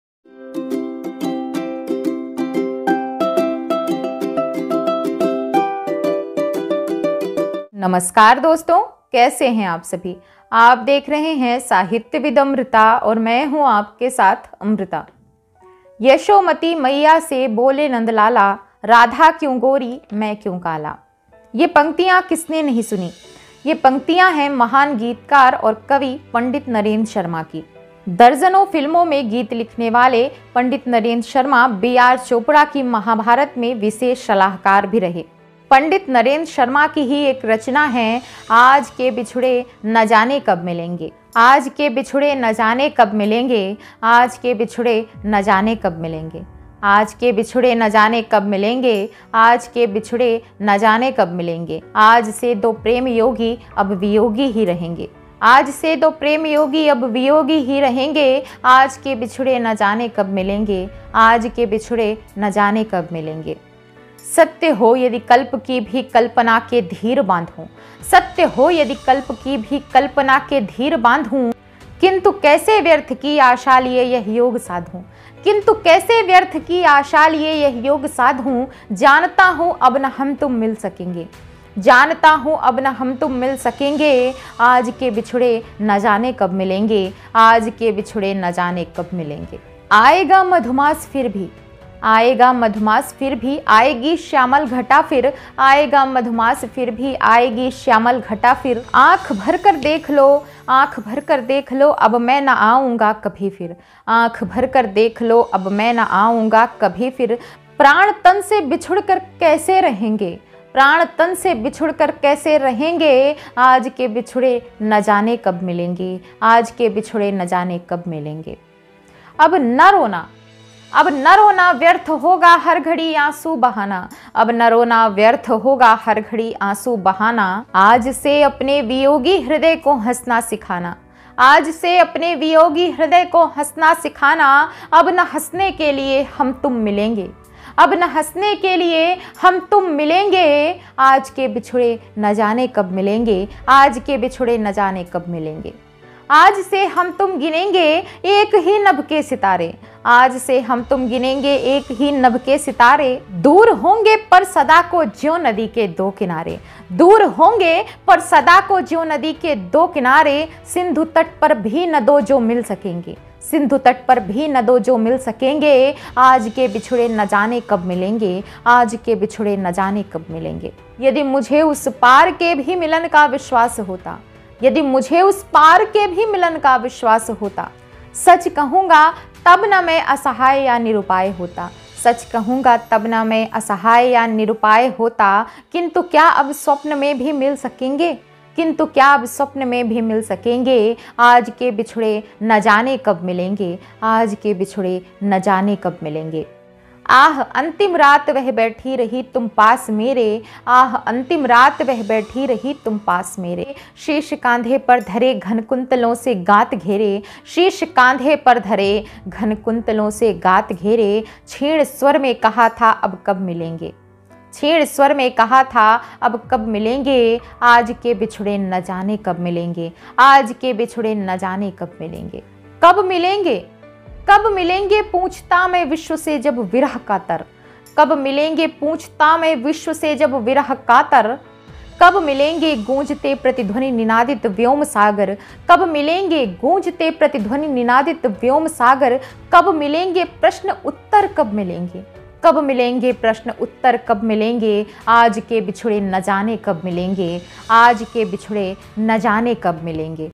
आज के बिछुड़े ना जाने कब मिलेंगे ? || Poetry By pandit narendra sharma
late shree" Pandit Narendra Sharma " was a great poet of india this video is a poetry video.this poetry "आज के बिछुडे ना जाने कब मिलेंगे " is written by great hindi poet "Pandit Narendra Sharma " which is like a star in hindi poetry.
all credit and copyright goes to late  "Pandit Narendra Sharma"

poetry narrated by "Amrita Pandey Pathak"

"पंडित नरेन्द्र शर्मा " जी हिन्दी कविता में तारे के समान हैं
प्रस्तुत है उनकी ओज कविता "आज के बिछुडे ना जाने कब मिलेंगे " कविता का वाचन कर रहीं हैं "अमृता पाण्डेय पाठक"